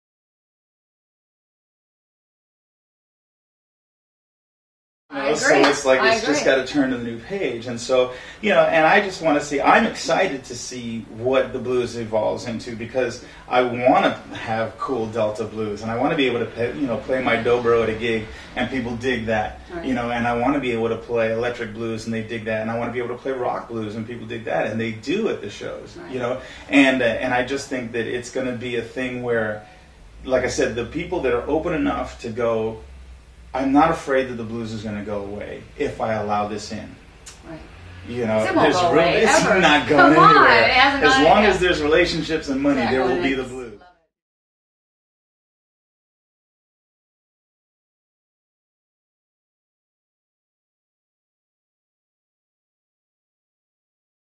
The Future of the Blues - Phil Gates - Interview @ Brickhouse Studios.mp4
Phil Gates tells Kelly Z his thoughts on what is in store for the blues..